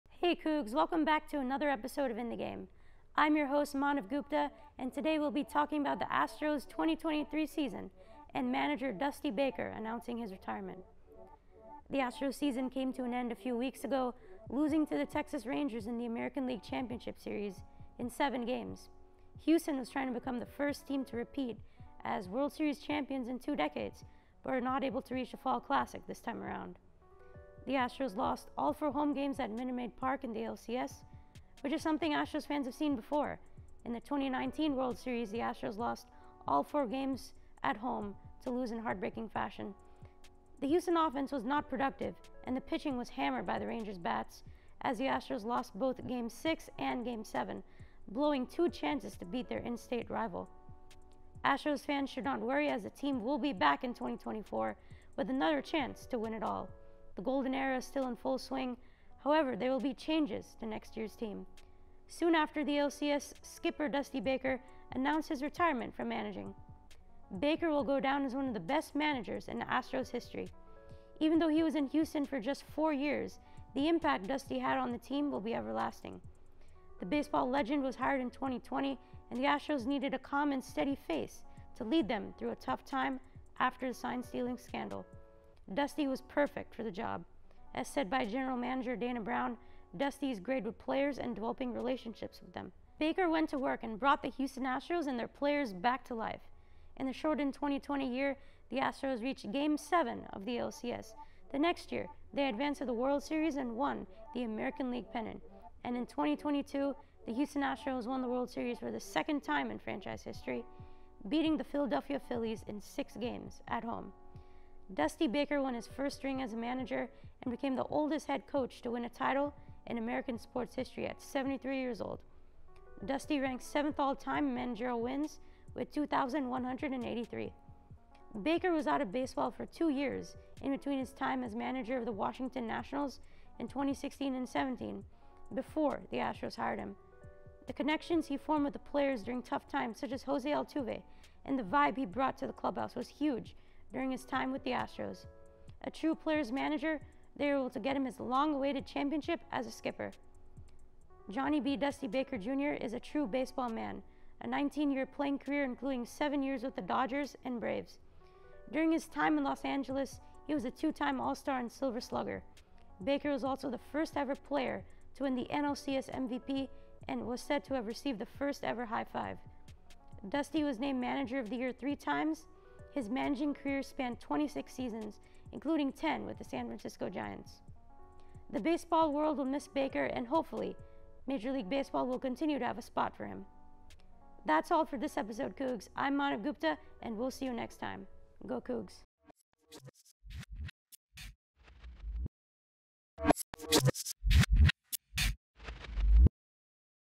Dusty Baker Retires I In The Game
Watch as In The Game host, Maanav Gupta, covers astros 2023 season and Dusty Baker's retirement.

Host: Maanav Gupta
Pre-Production Leader: Anthony Zavala
Crew: Jody Taller, Chirag Mangnaik, Daniel Rodriguez
Sports Producer: Lindsey Jimenez
Executive Producer: JoeMichael Coronado